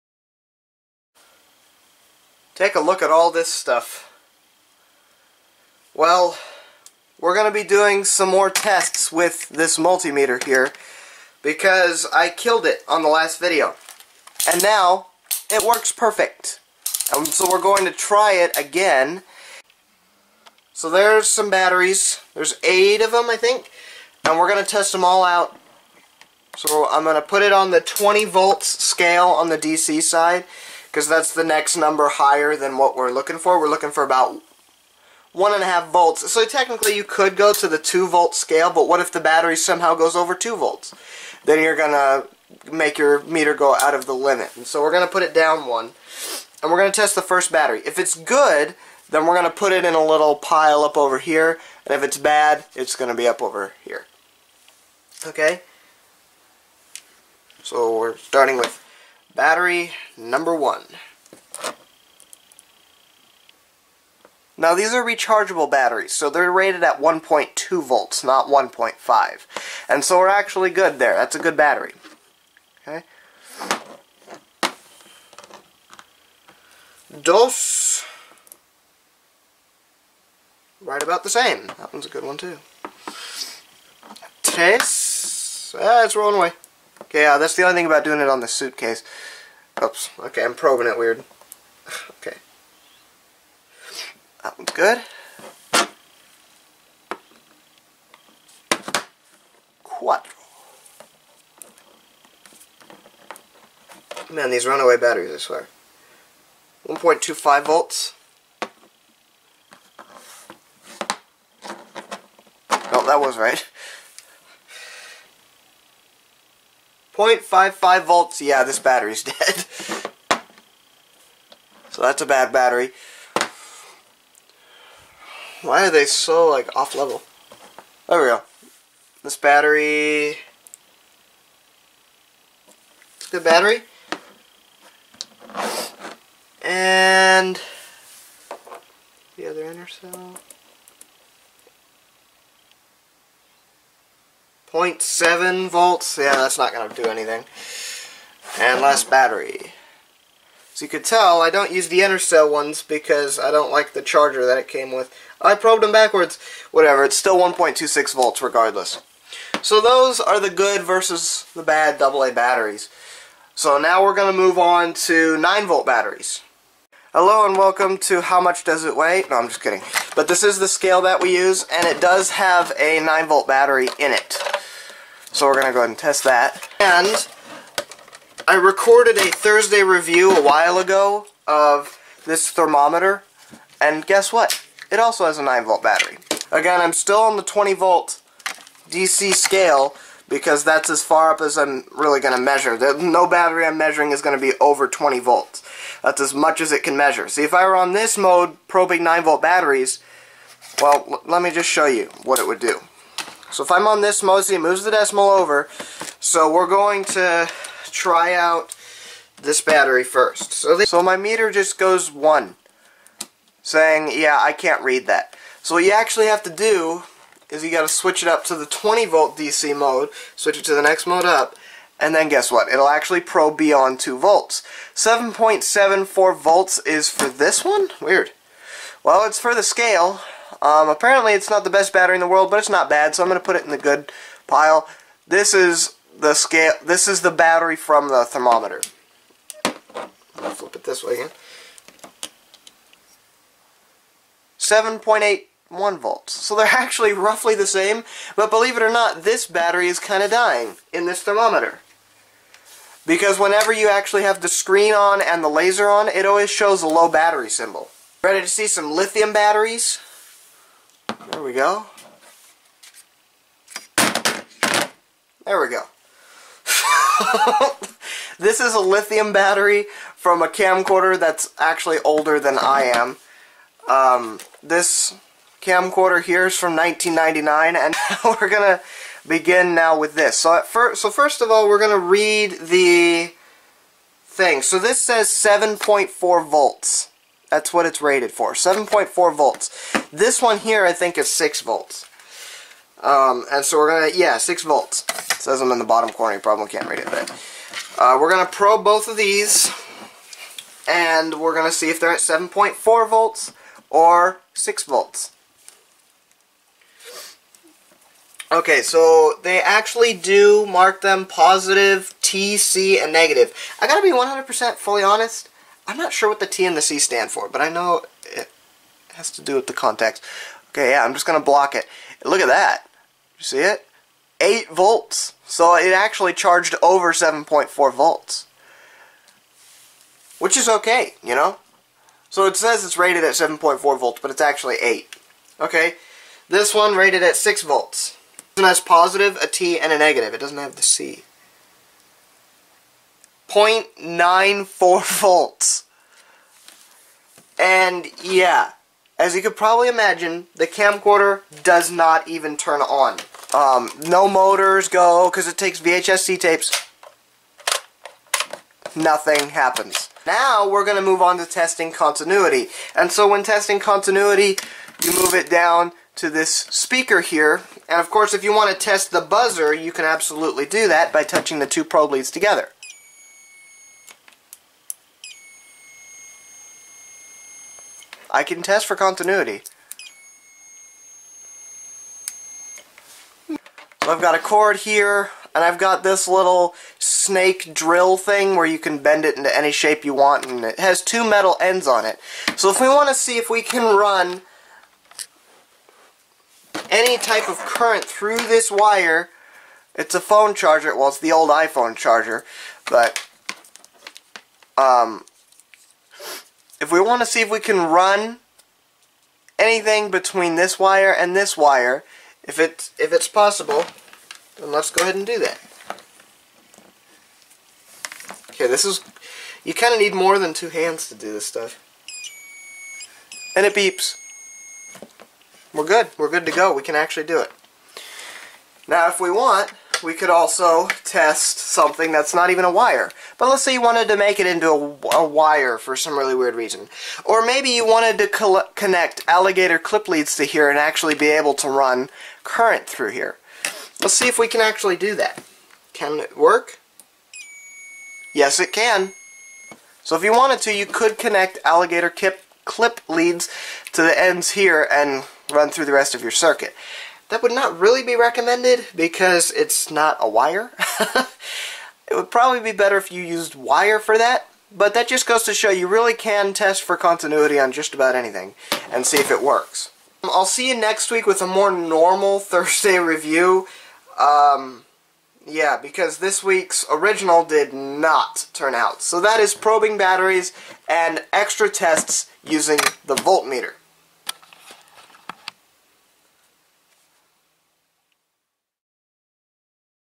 Thursday Reviews - Mastech Multimeter Tests (Ep. 19)
The model # for this meter is the MAS830. Here is a link to the original video of when I reviewed the meter